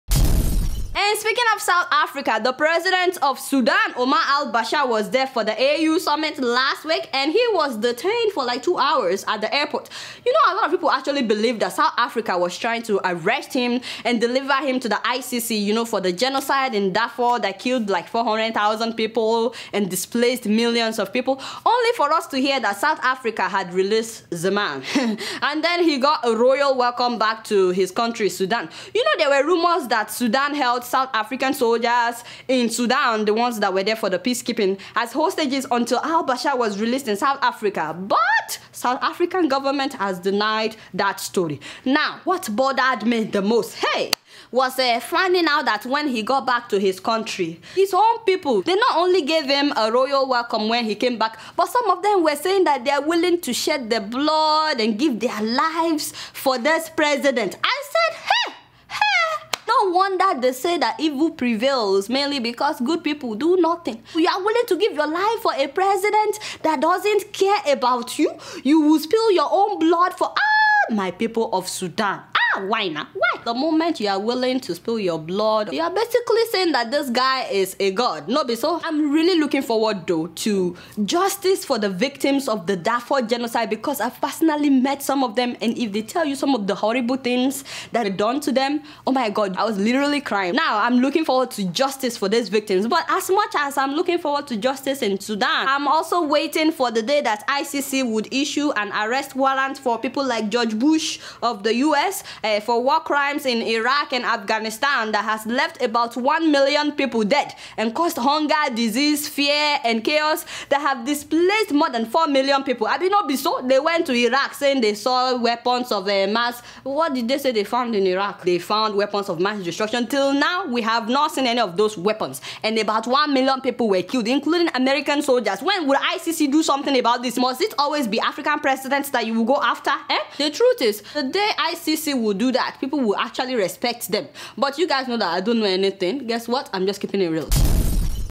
Omar Al-Bashir Escape Arrest In South Africa
In Sudan, President Omar Al-Bashir receives a honorary welcome after facing arrest briefly in South Africa.